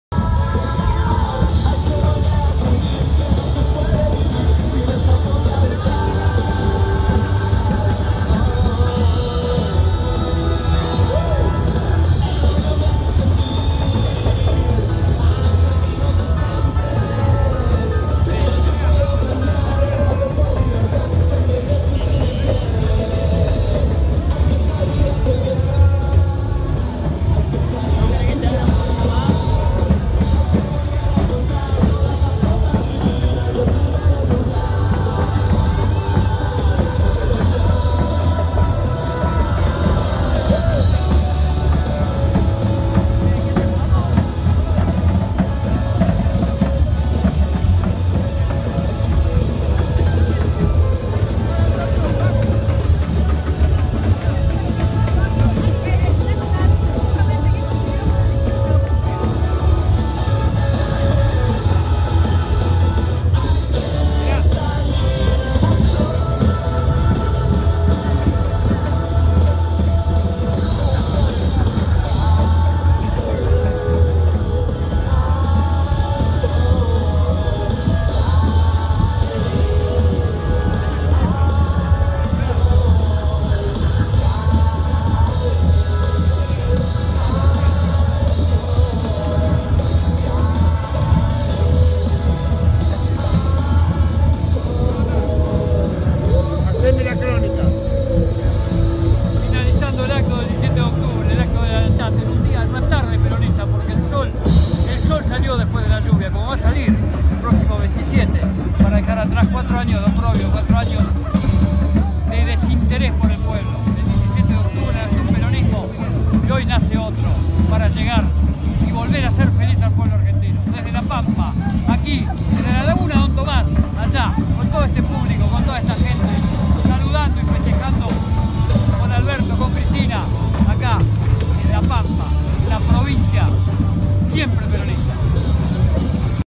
MULTITUDINARIO ACTO DEL DÍA DE LA LEALTAD EN LA PAMPA.
17 DE OCTUBRE.
2019.-

Cierre del acto en La Pampa.

Alberto Emaldi
Rodolfo Rocasalbas
Gustavo Ríos
Marcelo G. Cena

◾YT: Marcelo Cena PRENSA MAC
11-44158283